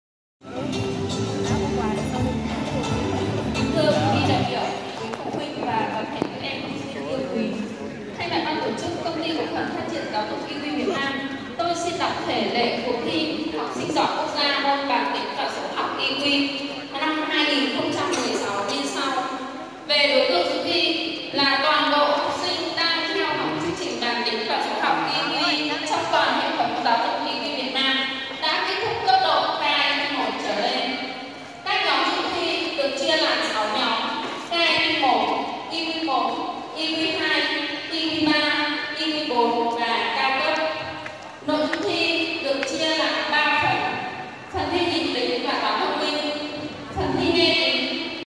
Cuộc thi bàn tính và IQ toàn quốc 2016(1)
Nguyễn Vũ Kỳ Anh: Nhất + cúp IQ2, 2016